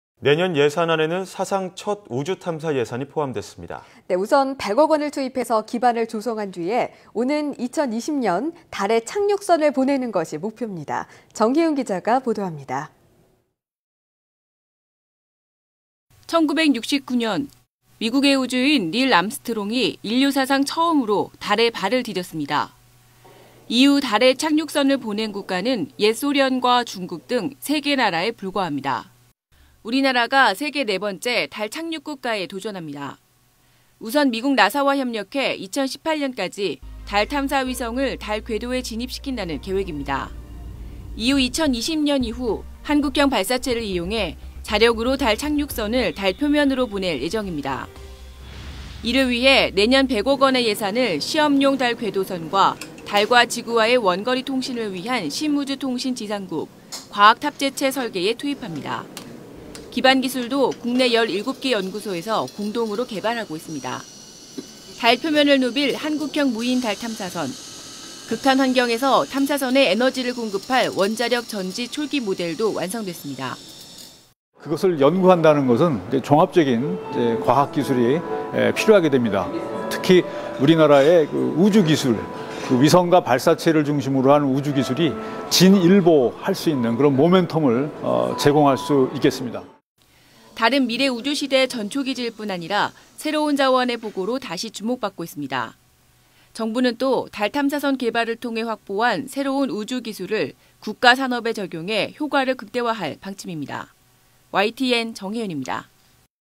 우주탐사에 100억 투입…"2020년 달 착륙" / YTN 사이언스
[앵커]
내년 예산안에는 사상 첫 우주탐사 예산이 포함됐습니다.

우선 100억 원을 투입해 기반을 조성한 뒤 오는 2020년, 달에 착륙선을 보내는 것이 목표입니다.

정혜윤 기자가 보도합니다.

[기자]
1968년 미국의 우주인, 닐 암스트롱이 인류 사상 처음으로 달에 발을 디뎠습니다.

이후 달에 착륙선을 보낸 국가는 소련과 중국 등 3개 나라에 불과합니다.

우리나라가 세계 4번째 달 착륙 국가에 도전합니다.

우선 미국 나사와 협력해 2018년까지 달 탐사 위성을 달 궤도에 진입시킨다는 계획입니다.

이후 2020년 이후 한국형 발사체를 이용해 자력으로 달 착륙선을 달 표면으로 보낼 예정입니다.

이를 위해 내년 100억 원의 예산을 시험용 달 궤도선과 달과 지구와의 원거리 통신을 위한 심우주통신 지상국, 과학탑재체 설계에 투입합니다.

기반 기술도 국내 17개 연구소에서 공동으로 개발하고 있습니다.

달 표면을 누빌 한국형 무인 달 탐사선.

극한 환경에서 탐사선에 에너지를 공급할 원자력 전지 초기 모델도 완성됐습니다.

[최기혁, 한국항공우주연구원 실장]
"달 탐사 장비를 연구한다는 것은 종합적인 과학 기술이 필요합니다. 특히 우리나라의 우주기술, 위성과 발사체를 중심으로 한 우주기술이 진일보할 수 있는 계기를 제공할 수 있습니다."

달은 미래 우주시대 전초기지일 뿐 아니라 새로운 자원의 보고로 다시 주목받고 있습니다.

정부는 또 달 탐사선 개발을 통해 확보한 새로운 우주기술을 국가 산업에 적용해 효과를 극대화할 방침입니다.

YTN 정혜윤입니다.